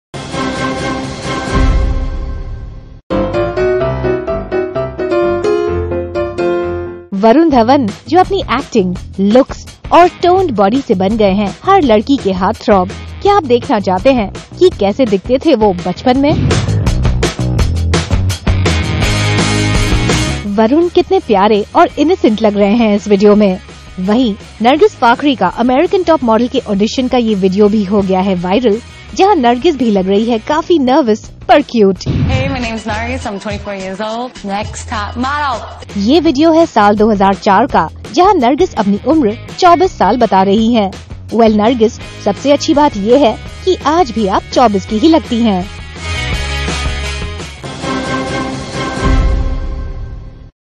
Why would Varun and Nargis shock you?
Varun Dhawan and Nargis Fakhri&rsquo;s hot bodies and looks have wooed many hearts. However, seeing them in this video might shock you!